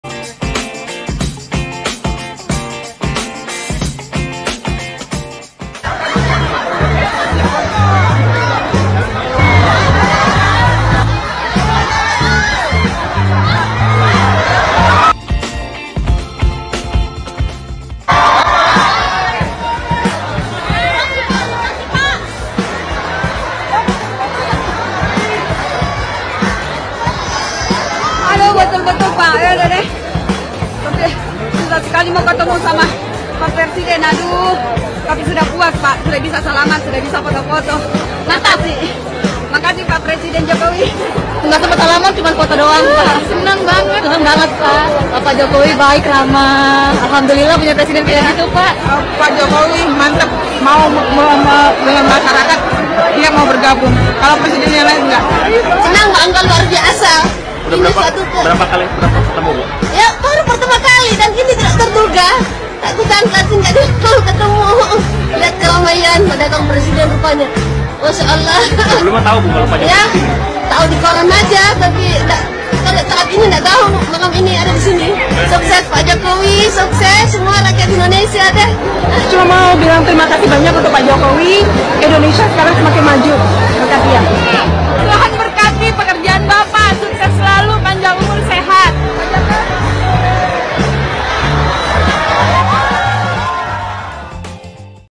Blusukan Jokowi Vs Prabowo
Yok kita lihat bagaimana blusukan para calon pemimpin negri ini. Apakah sedekat itu dengan rakyat, atau rakyatnya gak boleh dekat-dekat?

Silahkan disimak dan dikomentari.